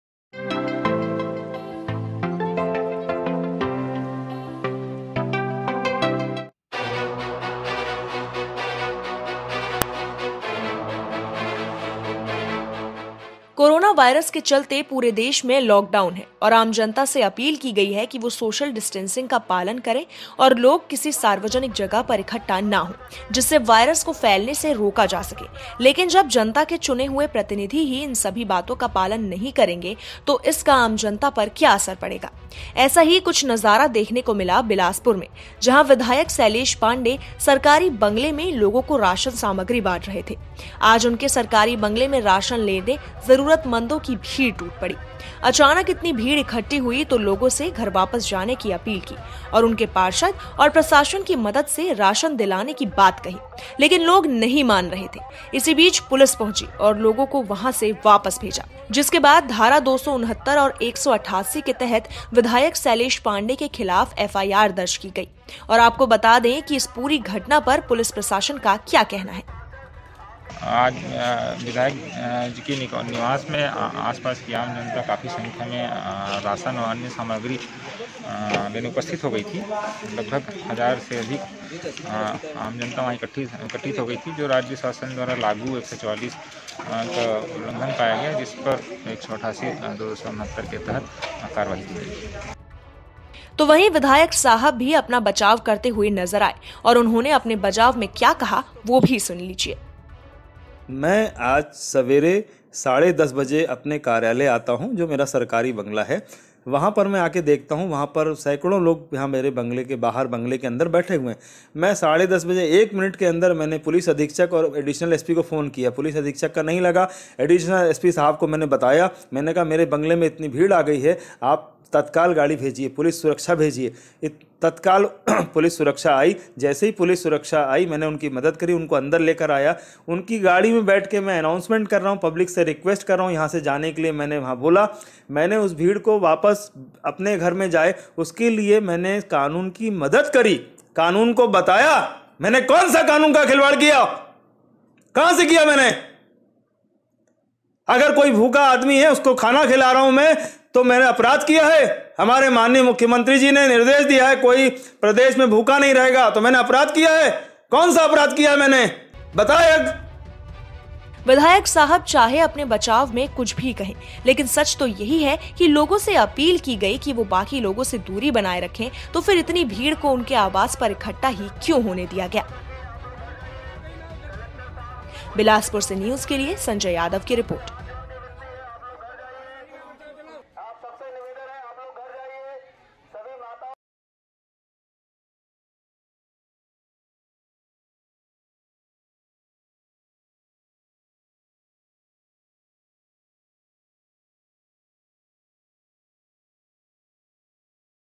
Bilaspur: Congress विधायक ने किया Lockdown का उल्लंघन, FIR हुई दर्ज
कोरोना वायरस के चलते पूरे देश में लॉक डाउन है और आम जनता से अपील की गयी है की वो सोशल डिस्टन्सिंग का पालन करें और लोग किसी सार्वजनिक जगह पर इकठ्ठा न हों. जिससे वायरस को फैलने से रोका जा सके. लेकिन जब जनता के चुने हुए प्रतिनिधि ही इन सभी बातों का पालन नहीं करेंगे तो इसका आम अजंता पर क्या असर पड़ेगा...ऐसा ही कुछ नज़ारा देखने को मिला बिलासपुर में जहाँ विधायक शैलेष पांडेय सरकारी बंगले में लोगों को राशन सामग्री बांट रहे थे, आज उनके सरकारी बंगले में राशन लेने जरूरतमंदों की भीड़ टूट पड़ी...